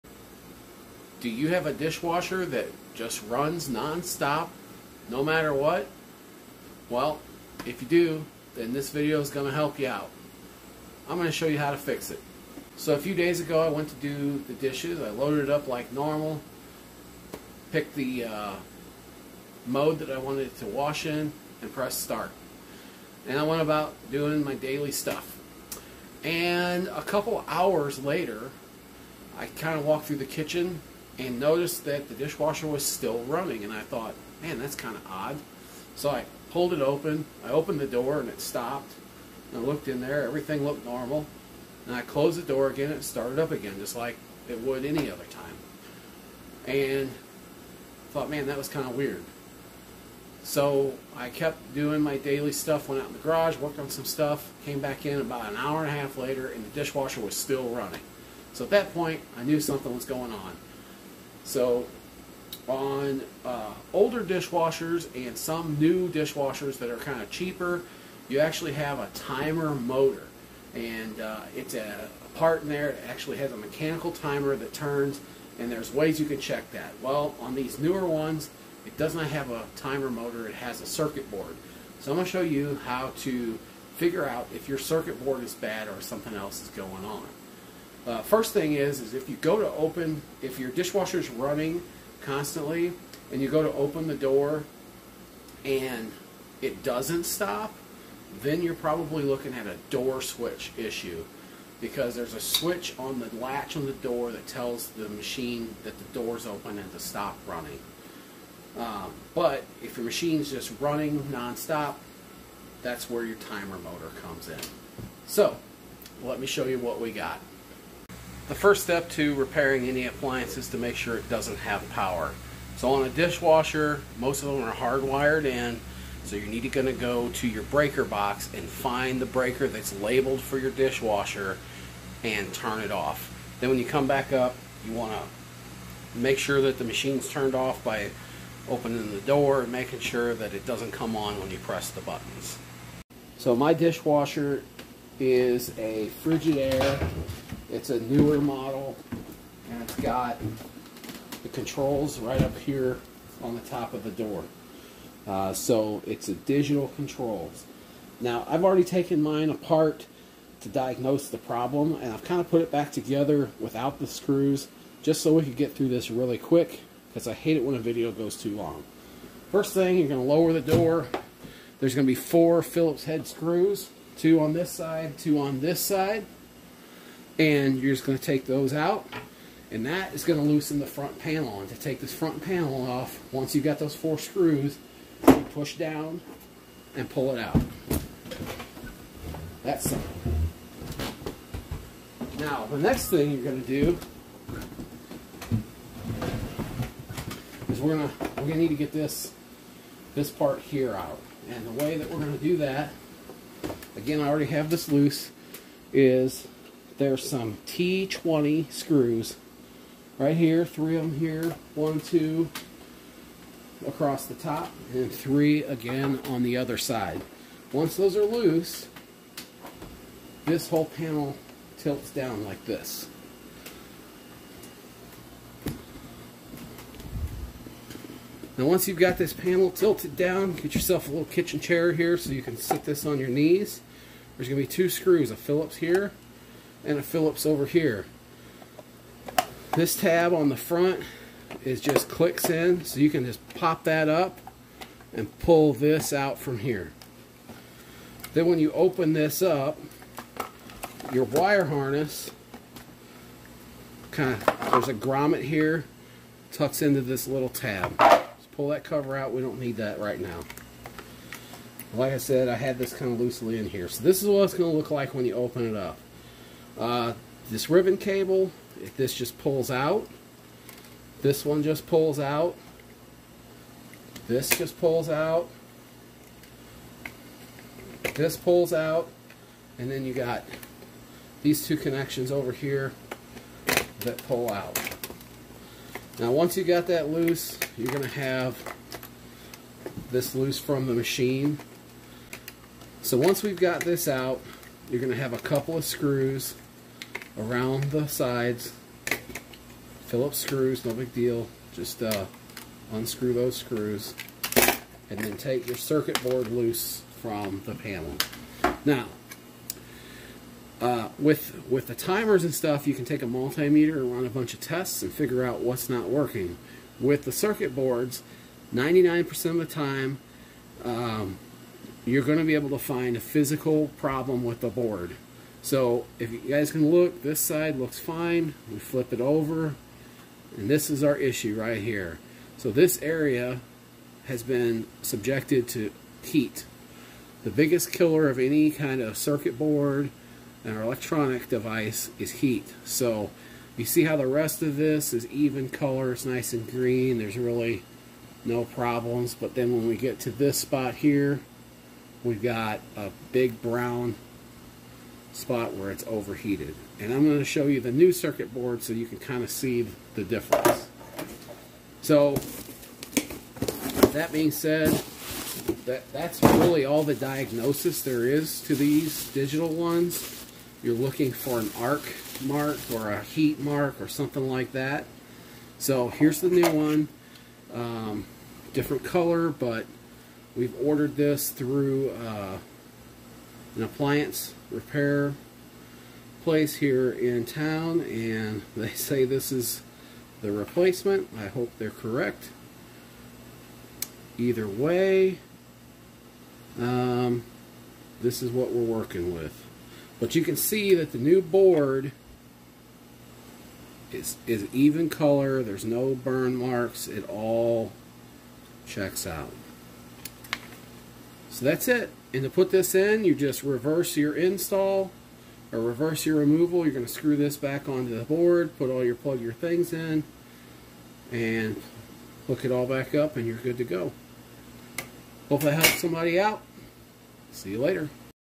How to fix a dishwasher that constantly runs
I fix my Frigidaire dishwasher that constantly runs. See how to remove and inspect the digital circuit board for damage.